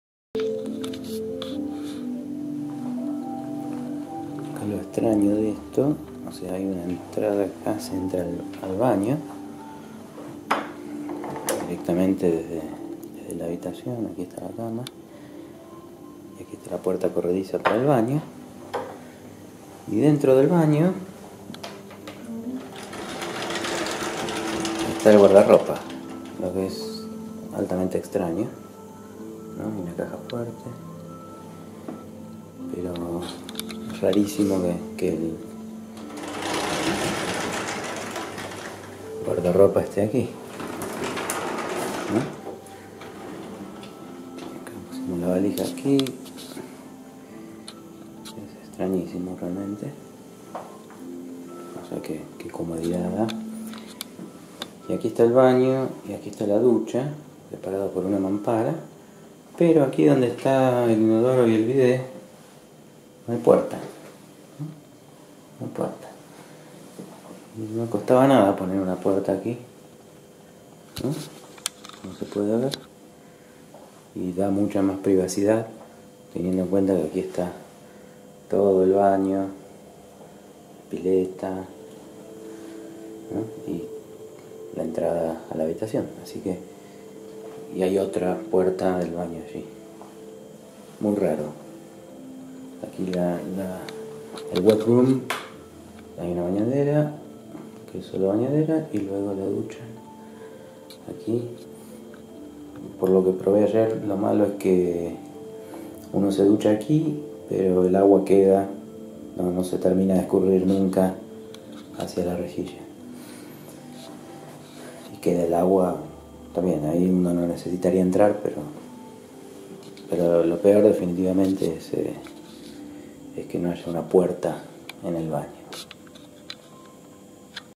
Park Hyatt Mendoza
Habitación doble común en el hotel Park Hyatt Mendoza. Extraña disposición del baño.